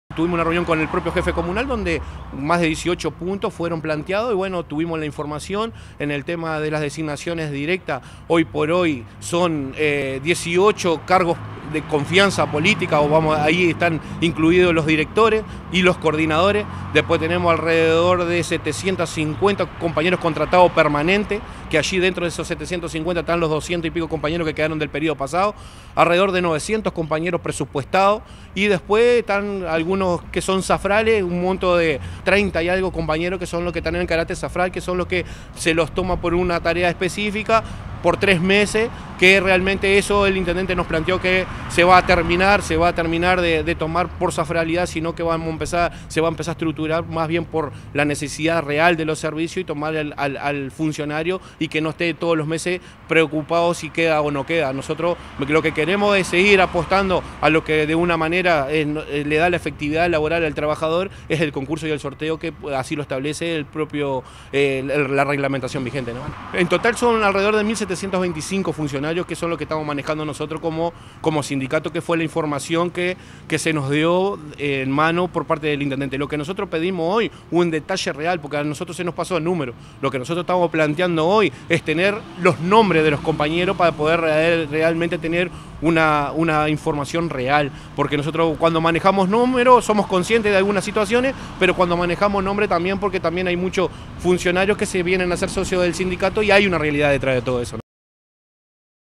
Plantilla municipal : 1725 funcionarios según datos de ADEOMS
Según datos que maneja ADEOMS, actualmente la Intendencia tiene 1725 funcionarios. De ellos, 18 son cargos de confianza política, 750 contratados permanentes, 900 presupuestados. Además hay trabajadores zafrales. Ésta información fue dada por la Intendencia al gremio.